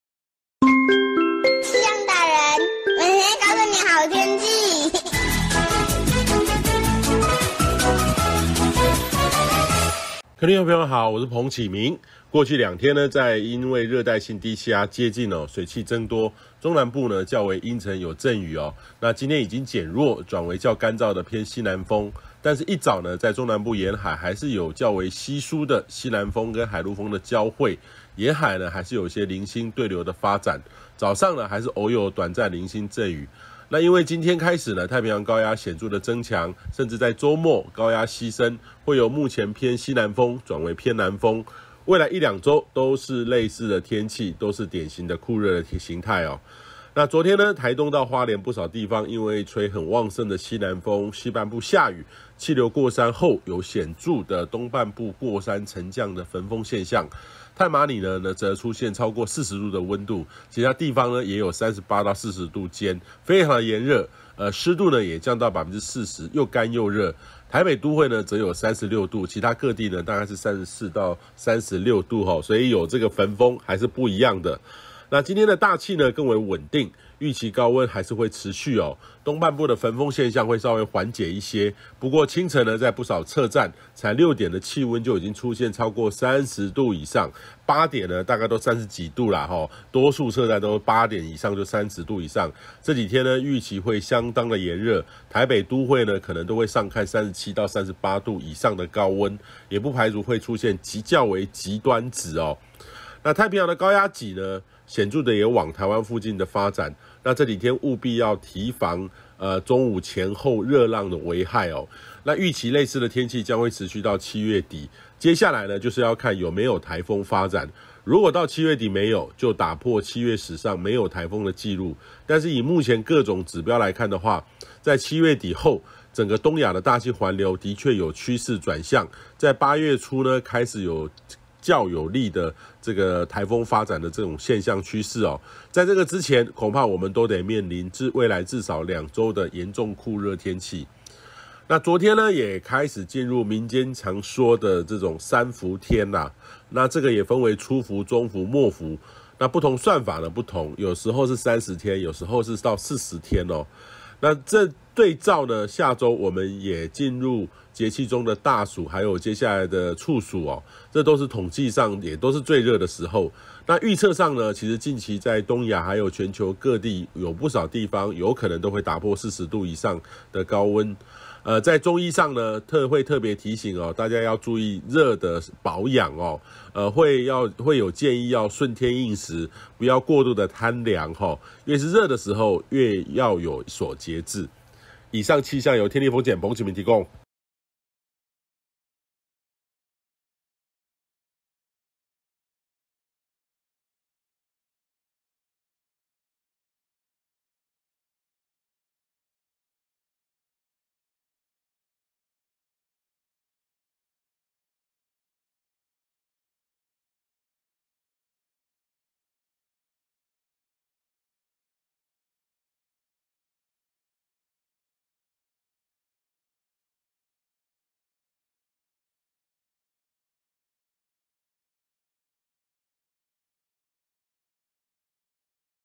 高壓增強轉趨炎熱 未來將連兩周熱浪天
過去兩天在熱帶性低氣壓接近，水氣增多，中南部較為陰沉有陣雨，今天已經減弱，轉為較乾燥的偏西南風，但一早在中南部沿海，還是有較為稀疏的西南風和海陸風的交會，沿海還是有些零星對流發展，早上還是偶有短暫零星陣雨。而今天開始太平洋高壓顯著的增強，甚至在周末，高壓西伸，會由目前偏西南風轉為偏南風，未來一兩周都是類似的天氣，都是典型的酷熱型態。

昨天台東到花蓮不少地方因為吹很旺盛的西南風，西半部下雨，氣流過山後有很顯著東半部過山沉降的焚風現象，太麻里出現超過40度的溫度，其他地方也有38-40度間，非常炎熱，濕度也降到40%，又乾又熱。台北都會則有36度，其他各地34-36度。

今天大氣更為穩定，預期高溫仍會持續，東半部的焚風現象會稍微緩解一些，不過清晨在不少測站，才六點多氣溫就已經出現超過30度以上，這幾天預期會相當的炎熱，台北都會可能都會上看37-38度的高溫，也不排除出現較為極端值。太平洋高壓的高壓脊，顯著的往台灣附近發展，這幾天務必要提防中午前後熱浪的危害。

預期類似的天氣將持續到七月底，接下來就是要看有沒有颱風發展，如果到七月底沒有，就打破七月史上沒有颱風的紀錄，但以目前各種指標來看，在七月底後，整個東亞的大氣環流，的確有趨勢轉向，在八月初開始會開始較有利於颱風發展的趨勢，在這之前，恐怕我們都得面臨未來至少兩周的嚴重熱浪天氣。

昨天也開始進入民間常常說的三伏天，這也分為初伏、中伏、末伏，不同算法不同，有時是30天，也有說是40天，今年7月16日入伏，8月24日出伏，共計40天，這對照下周我們也進入節氣中的大暑及接下來的處暑，都是統計上也是最熱的時候，而預測上，近期在東亞或全球各地有不少地方，可能都會出現40度以上的高溫。在中醫上，會特別提醒大家注意熱的保養，會建議要順天應時，不要過度貪涼，越是熱的時候越要有所節制。

以上氣象由天氣風險公司/彭啟明博士提供